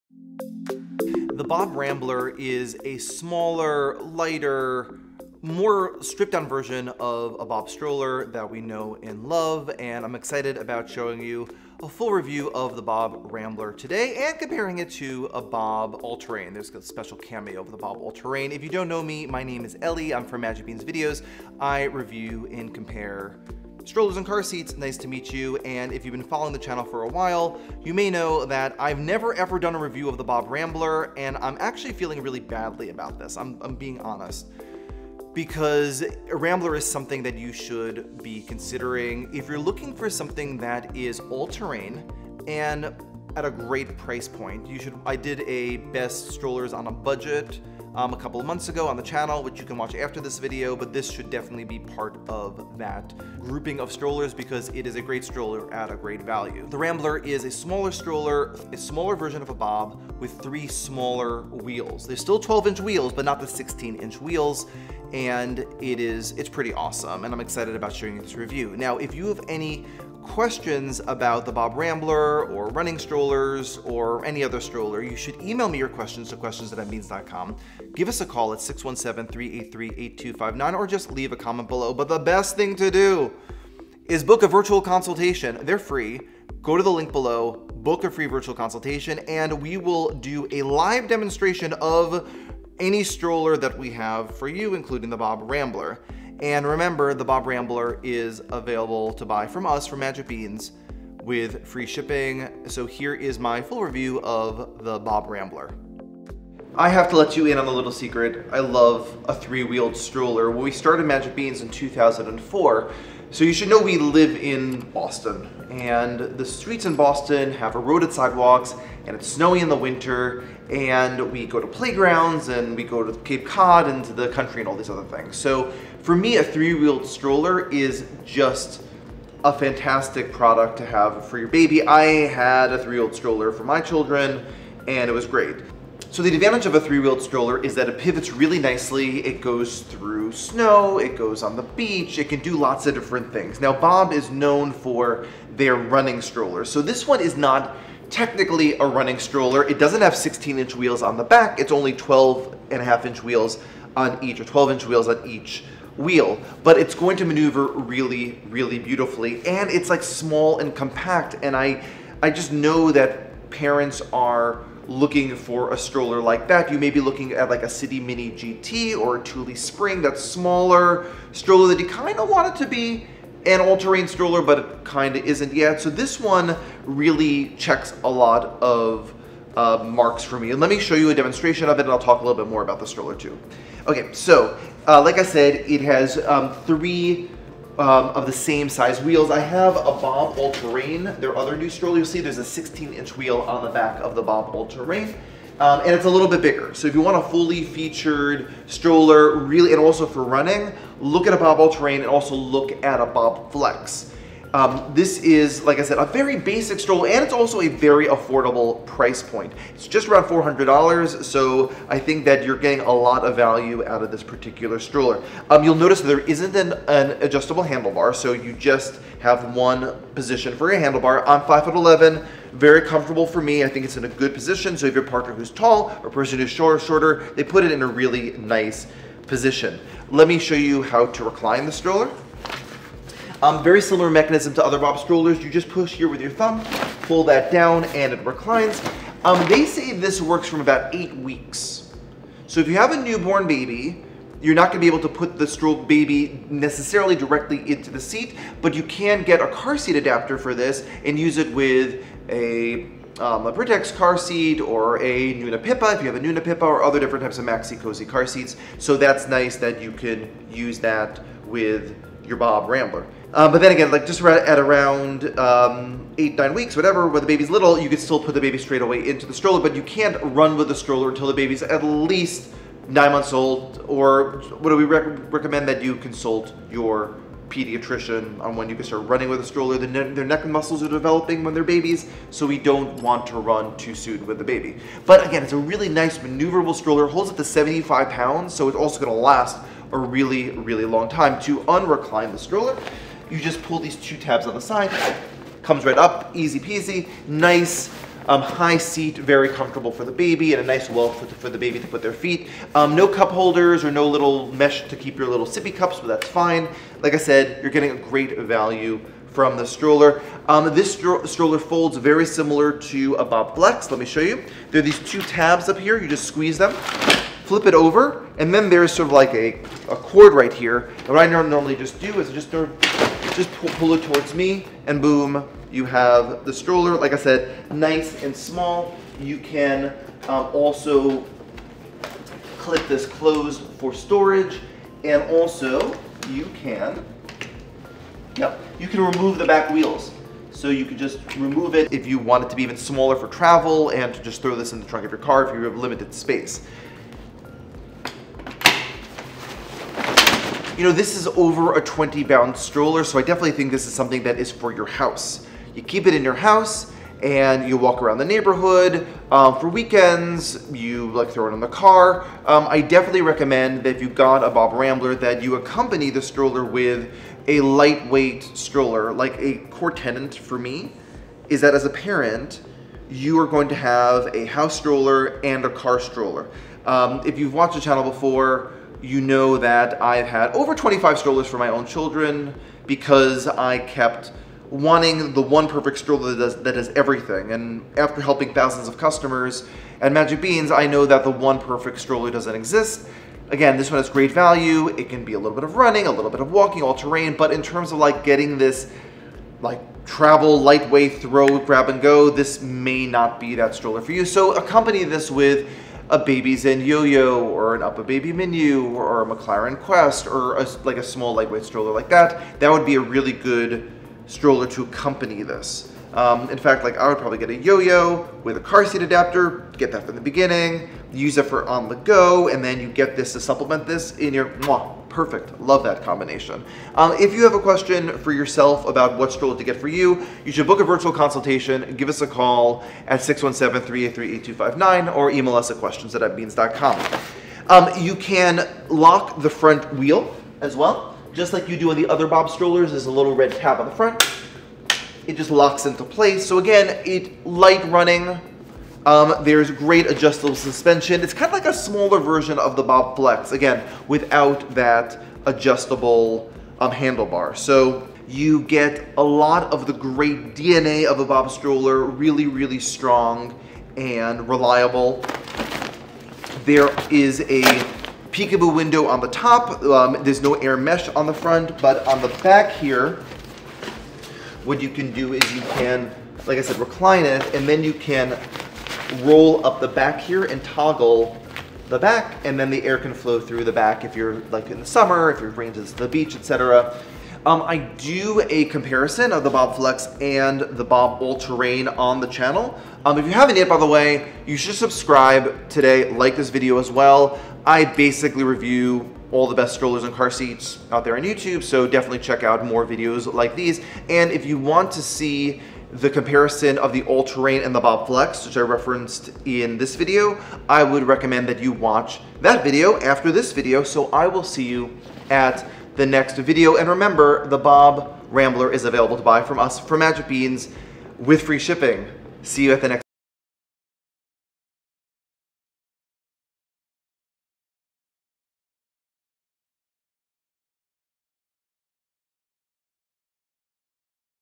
BOB GEAR Rambler Stroller | BOB GEAR Rambler Full Review | Magic Beans | Best Jogging Strollers
In need of a jogging stroller, but overwhelmed by their size? Look no further! Today, Eli will be demoing the BOB GEAR RAMBLER, the smaller, lighter, and more stripped down sister to the BOB ALTERRAIN PRO (which makes an appearance in this video!). Watch to see if the BOB GEAR RAMBLER is the right stroller for you!

Any questions? We'll match you with the perfect model for your family and lifestyle.